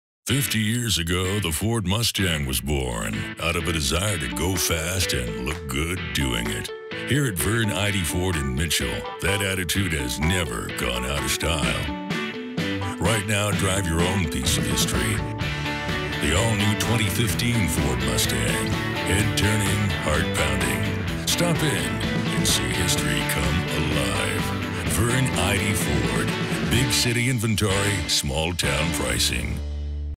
Find Your 2015 Ford Mustang at Vern Eide Ford in Mitchell, South Dakota
50 years ago, the Ford Mustang was born out of a desire to go fast and look good doing it. Here at Vern Eide Ford in Mitchell, that attitude has never gone out of style.